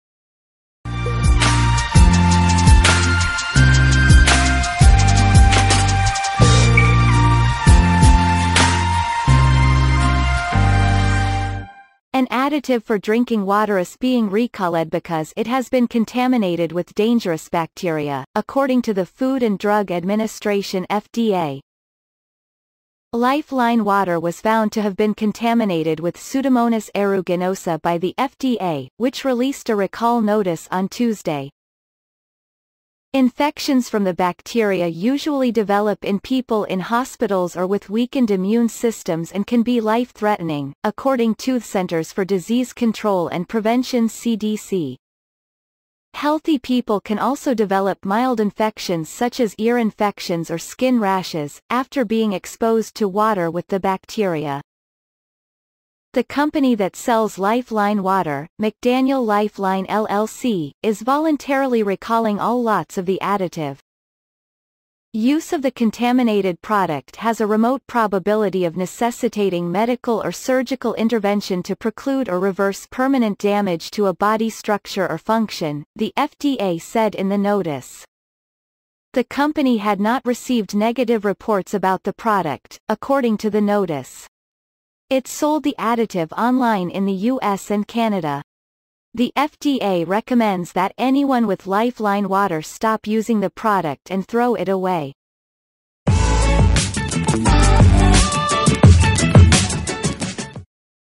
Water additive recalled over potentially deadly bacterial contamination
An additive for drinking wateris being recalledbecause it has been contaminated with dangerousbacteria, according to the Food and Drug Administration FDA.  Life-Line Water was found to have been contaminated with Pseudomonas aeruginosa by the FDA, which released a recall notice on Tuesday.  Infections from the bacteria usually develop in people in hospitals or with weakened immune systems and can be life-threatening, according totheCenters for Disease Control and Prevention CDC.  Healthy people can also develop mild...

Reference from FoxNews.